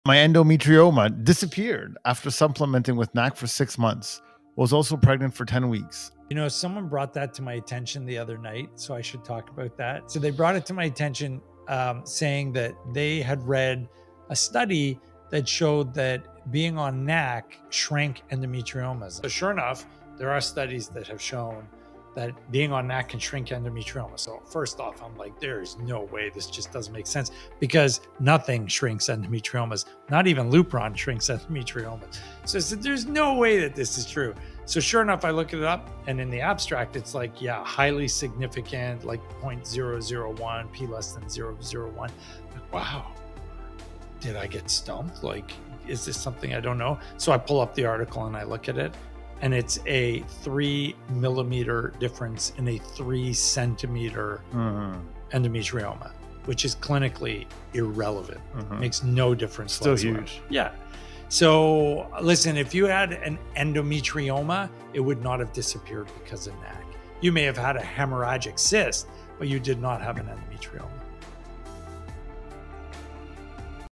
NAC Supplement Shrinks Endometriomas? The Science Behind It
NAC didn't shrink the endometrioma. A hemorrhagic cyst might look similar, but they are not the same. Don't be fooled.